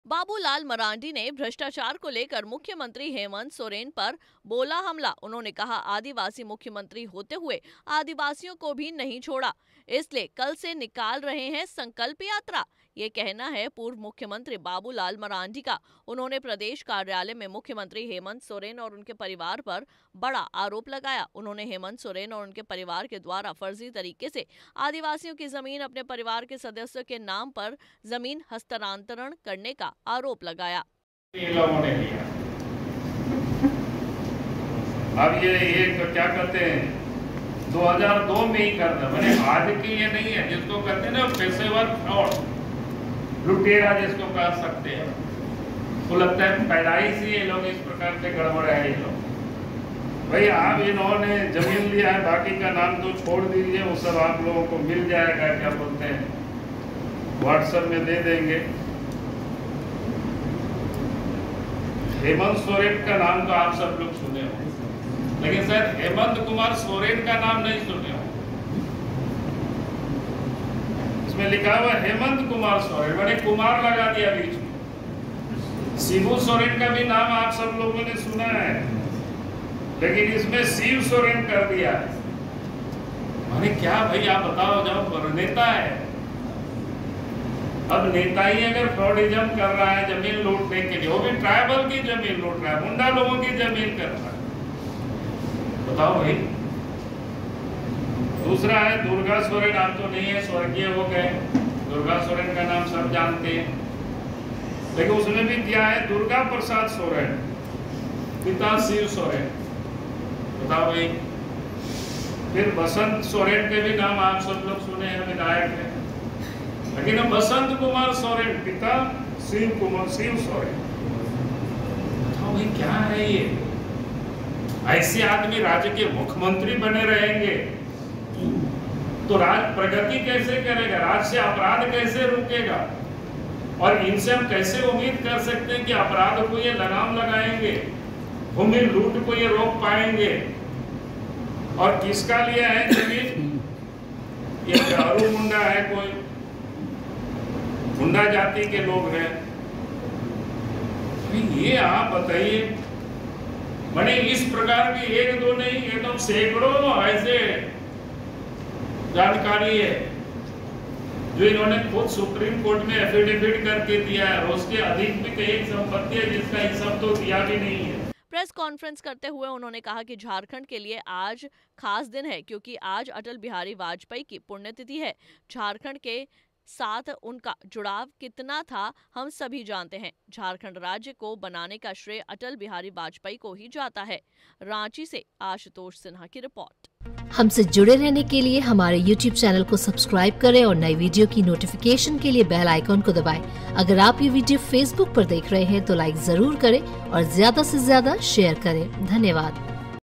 Babulal Marandi ने Hemant Soren पर लगाया बड़ा आरोप, पिता का नाम शिबू से कर दिया शिव
बाबूलाल मराण्डी(Babulal Marandi) ने भ्रष्टाचार को लेकर मुख्यमंत्री हेमन्त सोरेन(CM hemant Soren) पर बोला हमला... उन्होंने कहा, आदिवासी मूख्यमंत्री होते हुए... आदिवासियों को भी नही छोड़ा, इसलिए कल से निकाल संकल्प यात्रा रहे हैं. ये कहना है पूर्व मुख्यमंत्री बाबूलाल मरांडी का..